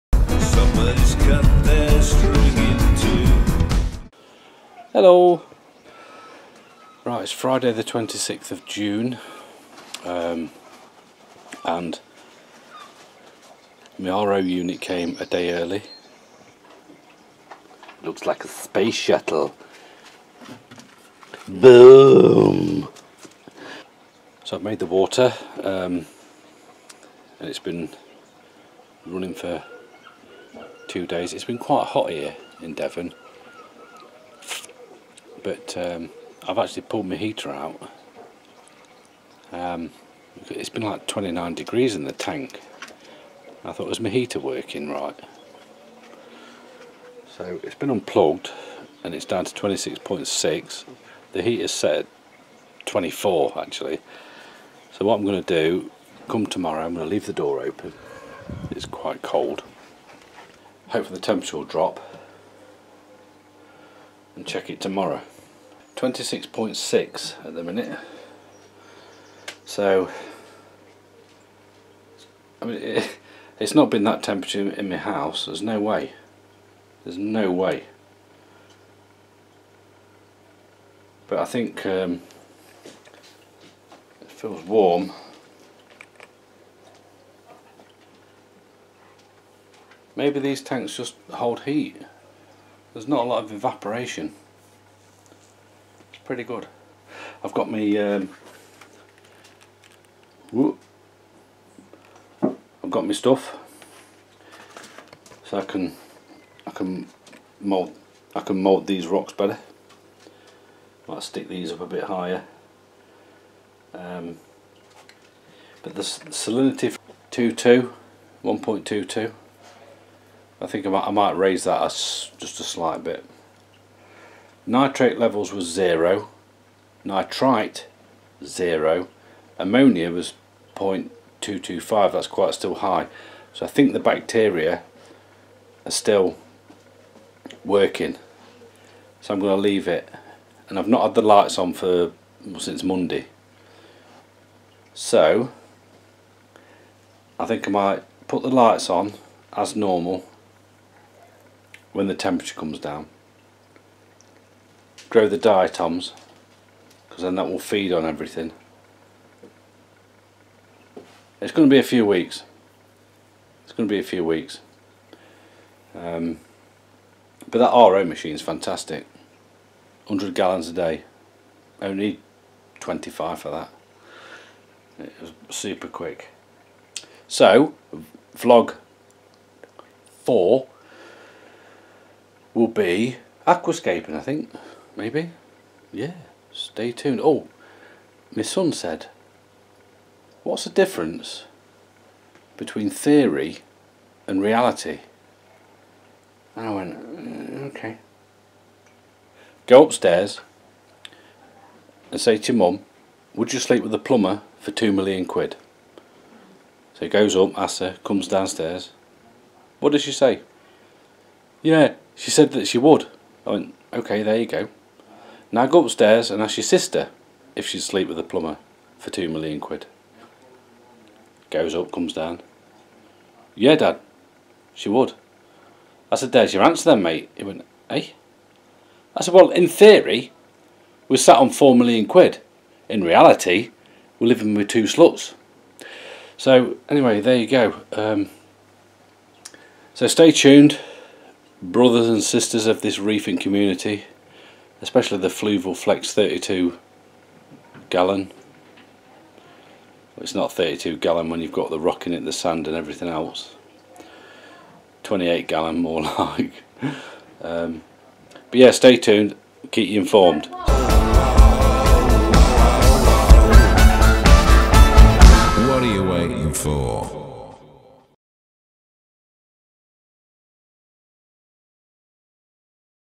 Fluval Flex 123 L | Vlog 3
RODI unit installed...water in...jokes...dad jokes...lol